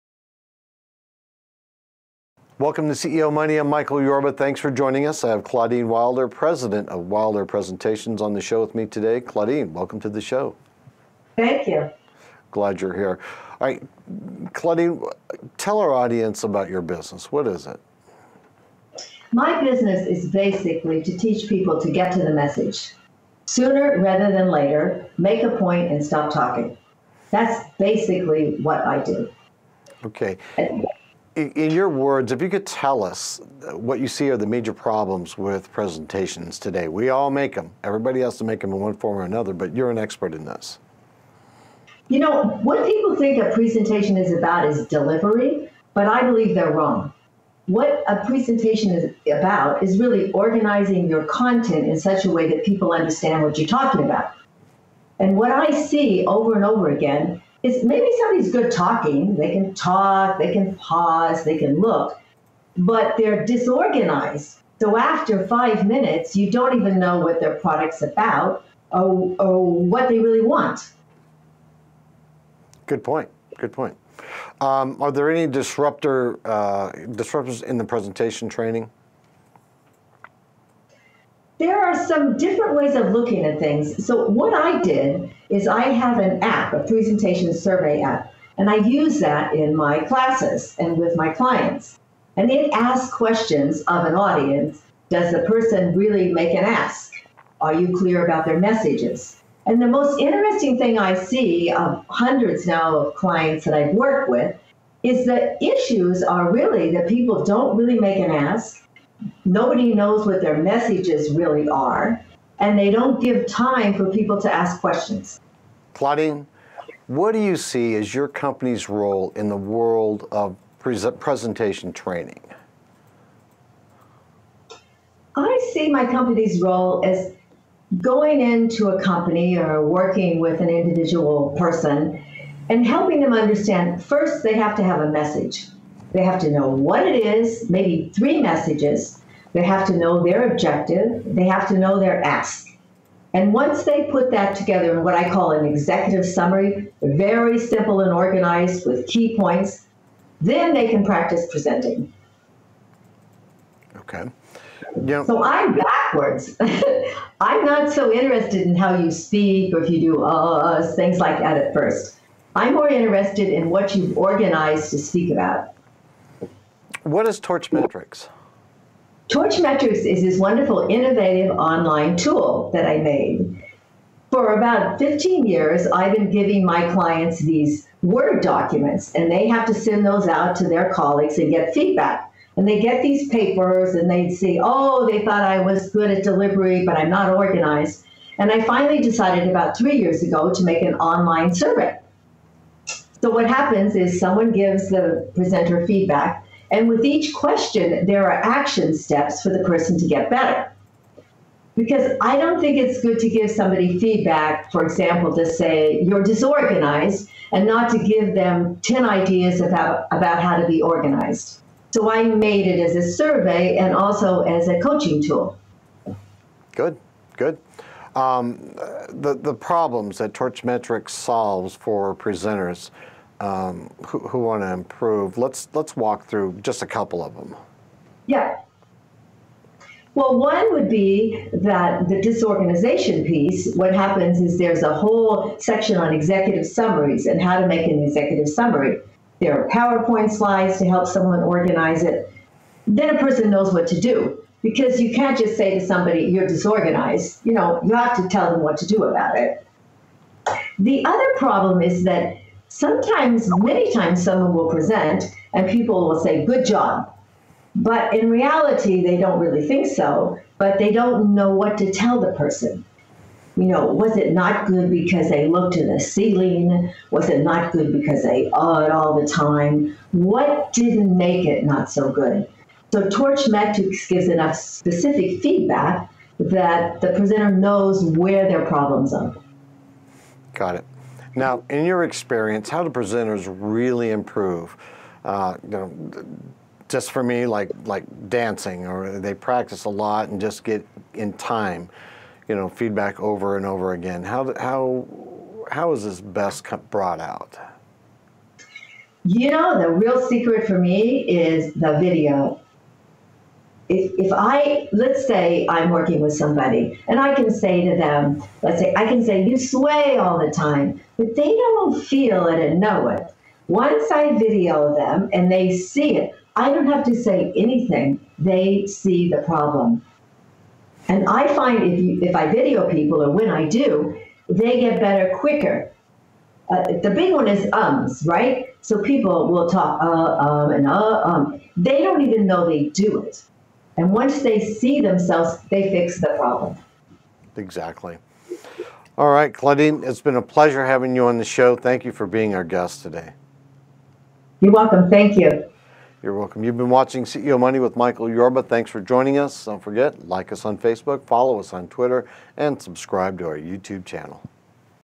Claudyne Wilder coaches people in to overcome obstacles to better business presentation skills
Claudyne Wilder is the president of Wilder Presentations, a niche boutique presentations company. Since 1998 Claudyne Wilder’s Get to the Message Presentation Workshops has been taught in companies from Fortune 500 pharmaceutical firms to community-based non-profits. Claudyne has been an innovator in the presentation world. She was the first to write a book about laptop presenting. She co-conducted one of the first user surveys on PowerPoint productivity.
 She now has a unique online presentation survey and coaching tool called TorchMetrics. Effectively it is your own personal speaking coach and career builder in the palm of your hand.

Claudyne Wilder
President
Wilder Presentations